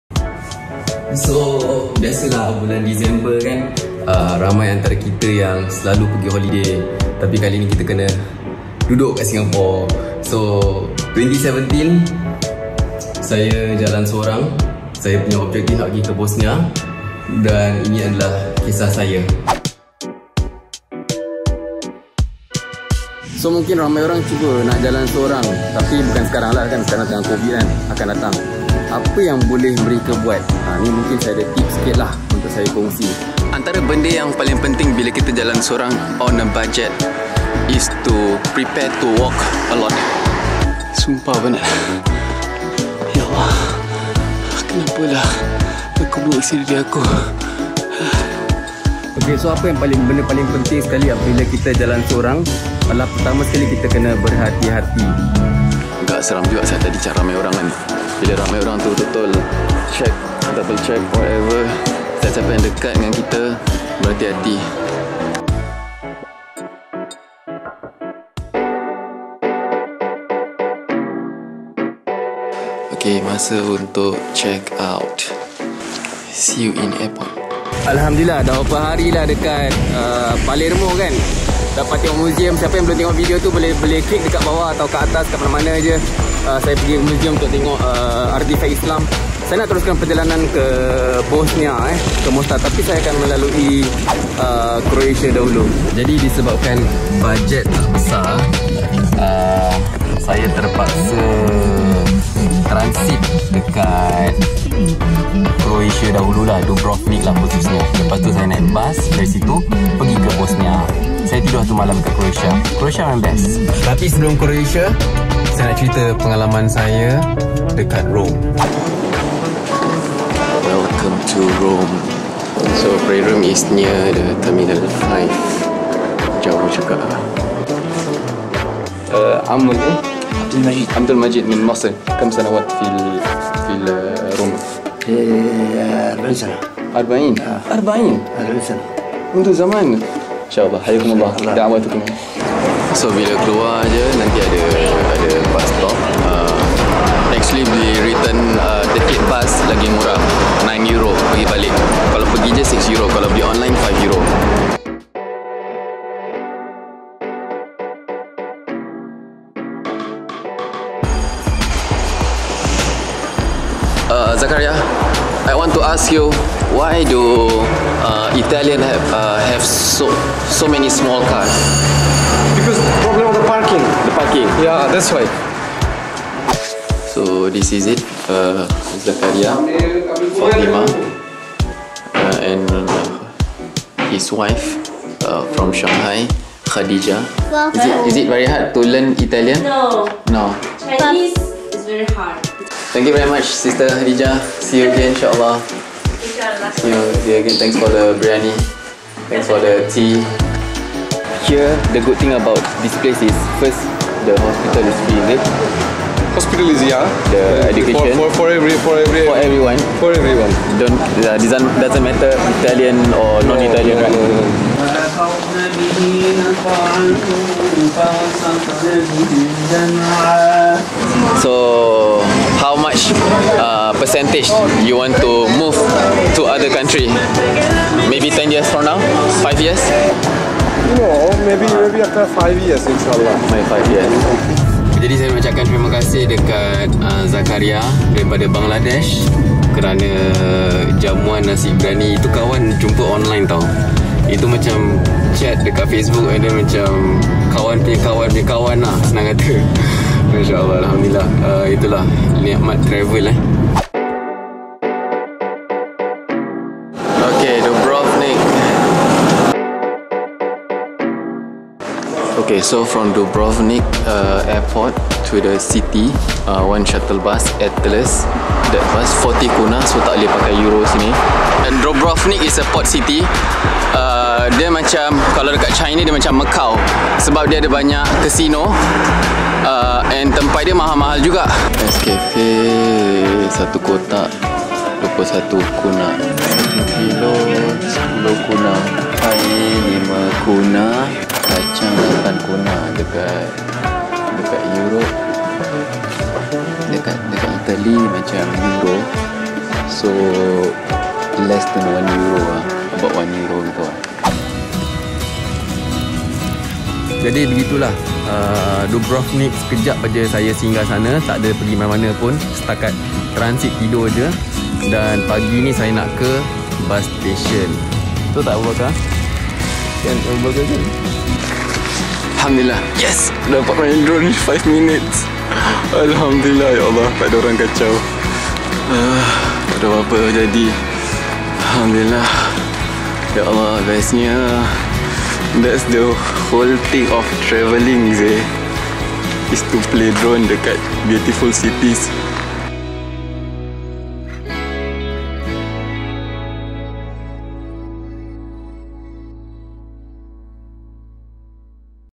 Tips travel alone from Palermo, Italy to Dubrovnik, Croatia via Rome (in Malay)
Pengalaman 2017 Jalan Sorang dari Italy ke Bosnia via Croatia. Di Rome, Italy saya jumpa dengan sahabat, Zakaria dari Bangladesh. Cari Surau di Airport Rome. Nikmat Islam, nikmat sahabat.

Sedikit tips jalan sorang.
Sedikit review harga-harga barang di Dubrovnik (Croatia)

Sila share pengalaman Jalan Sorang anda di comment 👍